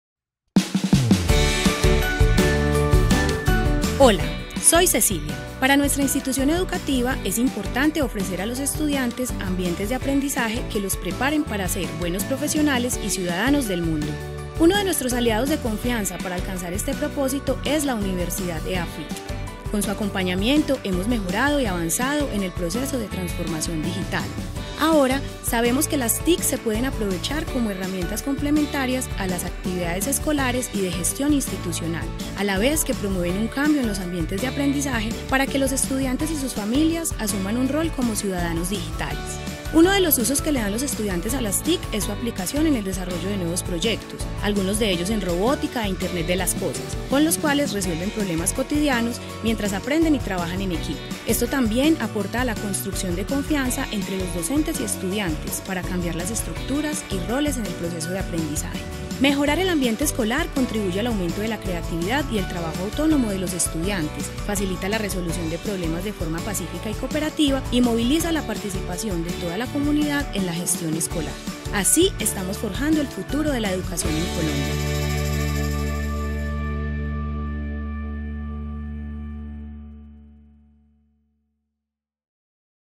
Tecnologías digitales como aliadas de la transformación escolar
El uso continuo de las TIC en ambientes de aprendizaje permite concebirlas como herramientas complementarias que ayudan al desarrollo de ideas renovadas, a la movilización de la creatividad de los estudiantes y a la vinculación de la comunidad educativa en el proceso de consolidación de una cultura de innovación mediada por TIC.